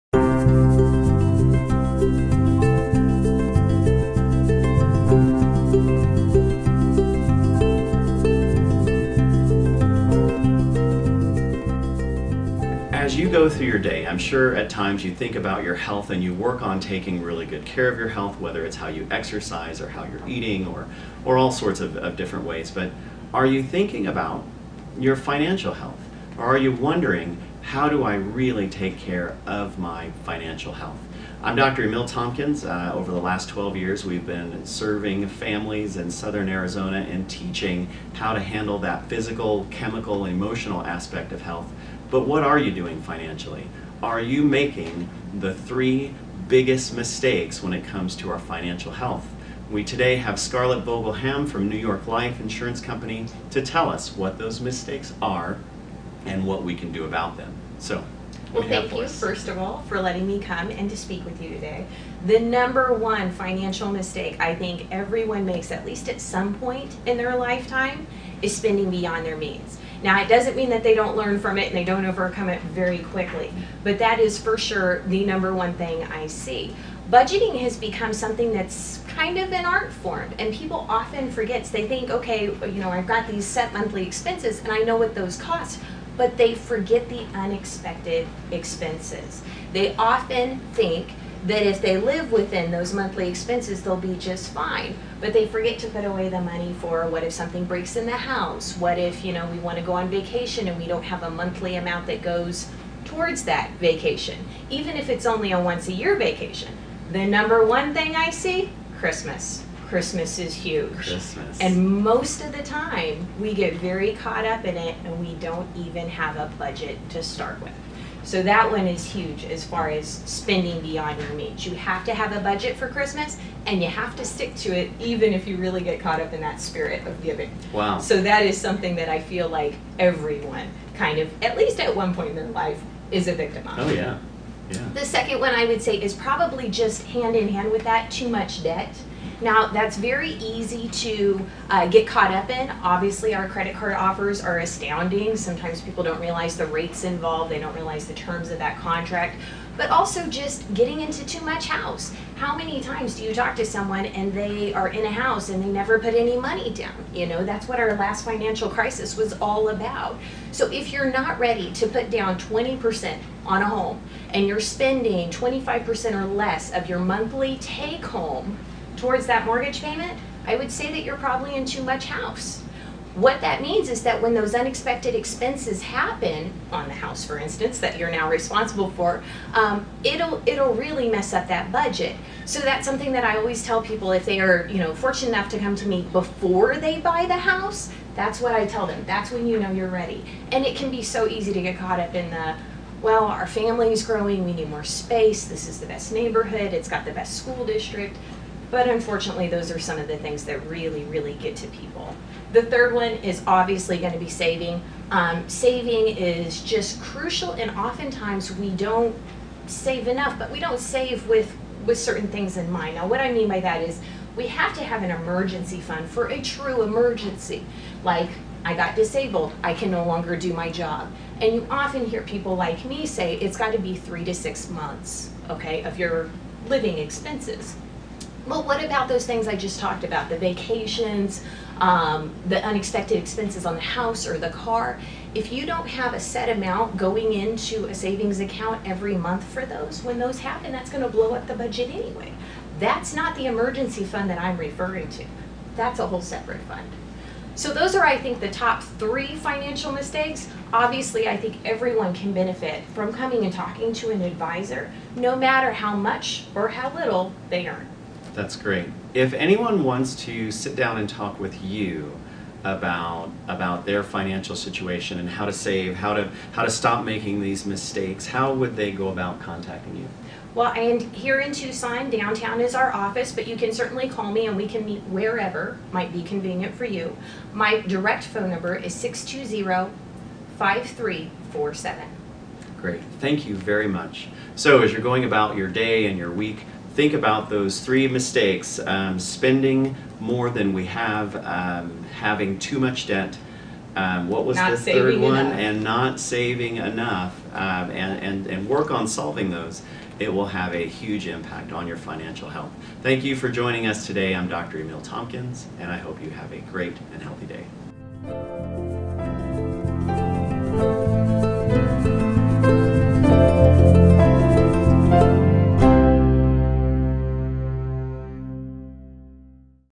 3 Biggest Financial Health mistakes you can make.
Dr. Emil Tompkins and Scarlett Vogel Hamm talk about how we sabatoge our own financial health.